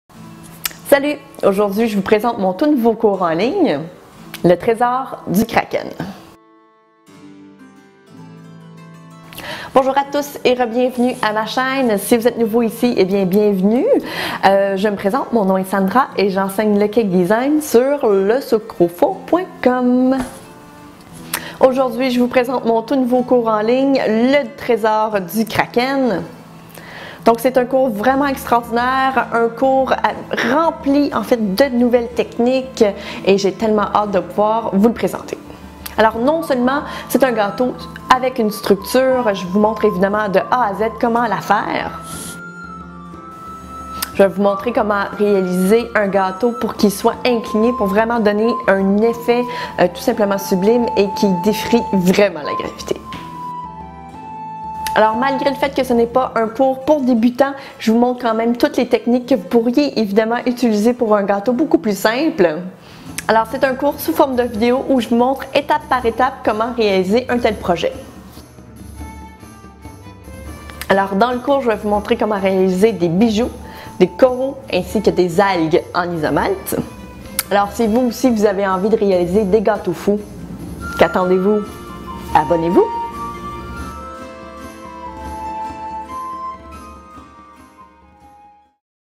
Cours de cake design en ligne le trésor du kraken
Dans ce cours je vous explique de A à Z comment réaliser ce projet. Apprenez à réaliser un gâteau 3D, avec une structure que vous pourrez faire vous même, comment réaliser un effet de bois, des coraux, des bijoux et des algues en isomalt et plus encore.

Je vous donne la liste des outils que j'ai utilisés pour réaliser ce projet ainsi que des shémas avec tout les mesures et les indications pour que vous puissiez le recréer facilement.

Le cours est d'une durée de 5h et est divisé en étapes.

dès le 1er avril